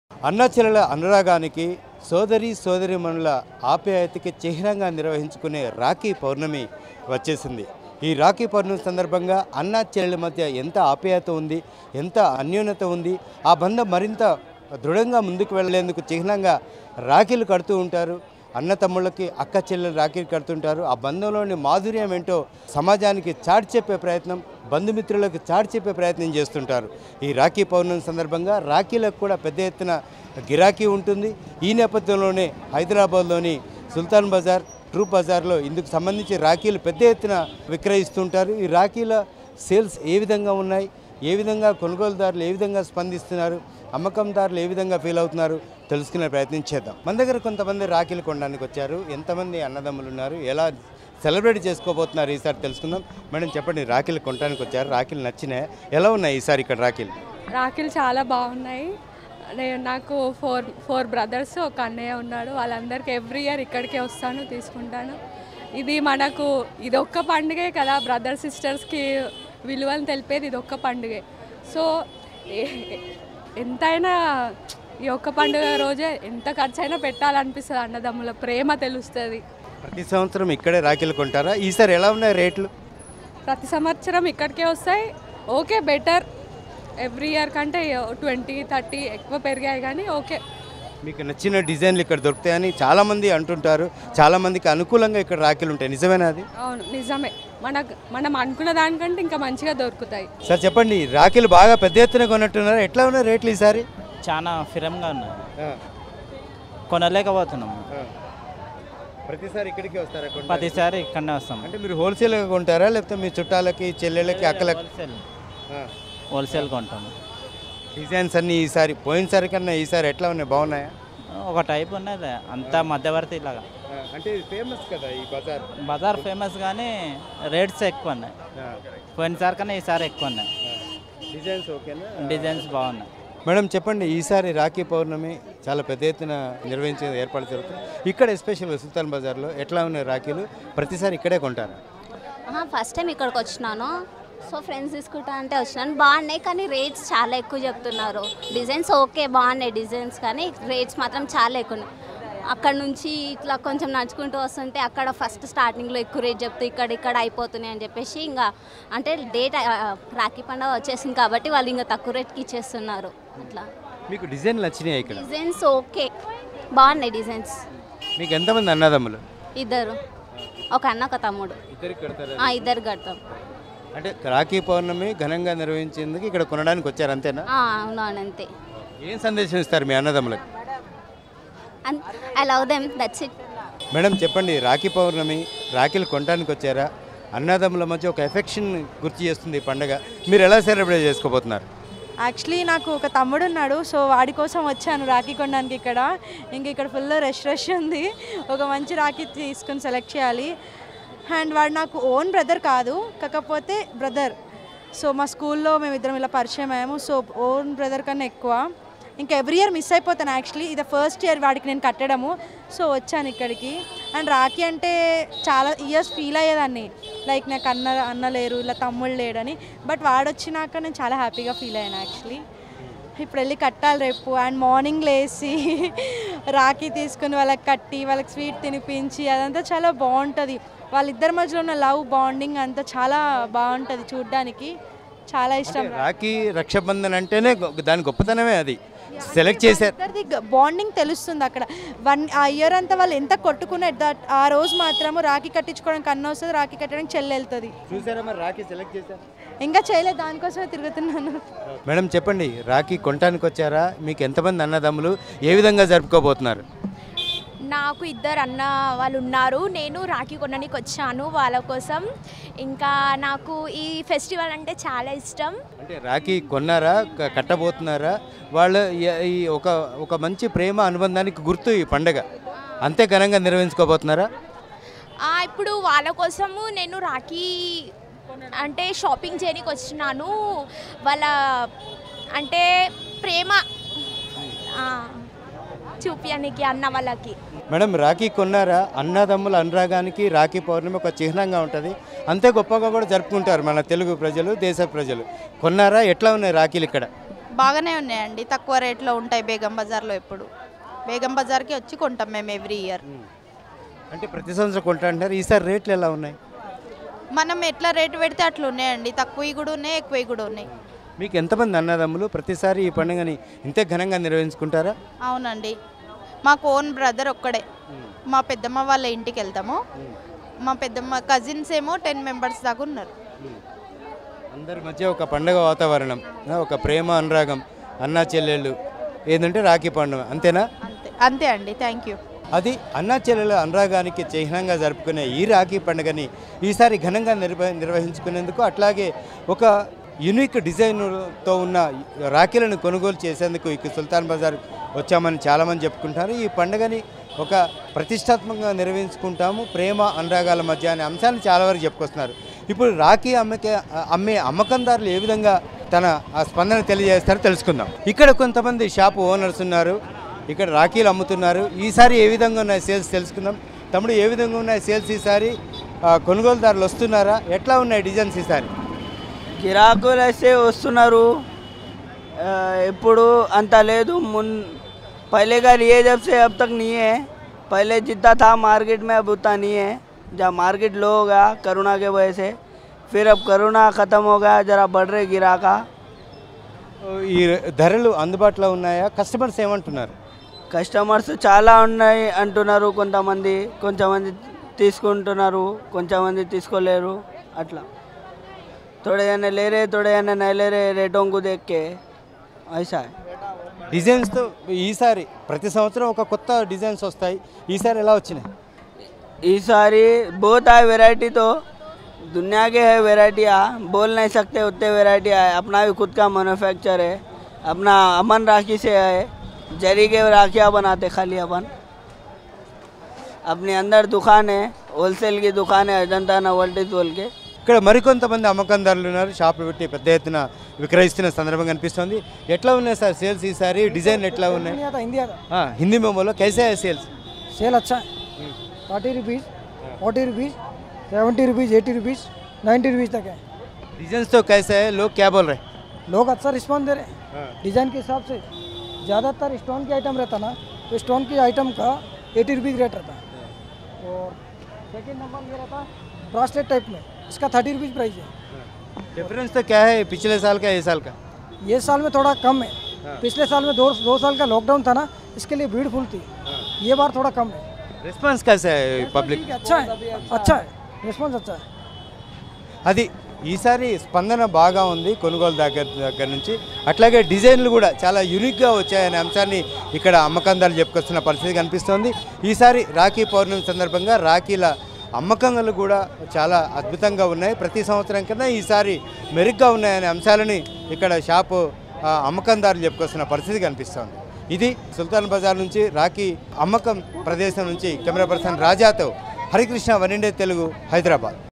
Raksha Bandhan 2022...హైదరాబద్ లో ఘనంగా రాఖీ పండుగ *Telangana | Telugu OneIndia
Rakhi Sales in Sultan bazar, Hyderabad | హైదరాబద్ లో ఘనంగా రాఖీ పండుగ..సుల్తాన్ బజార్ లో రాఖీ సేల్స్ ఇలా...